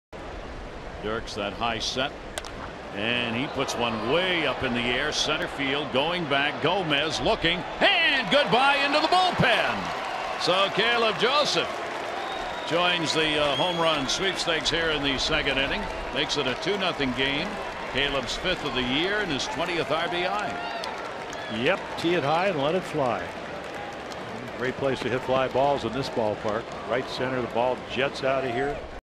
BAL@TEX: Joseph belts a solo homer to left-center
Caleb Joseph drives a solo home run to left-center field to give the Orioles a 2-0 lead in the top of the 2nd inning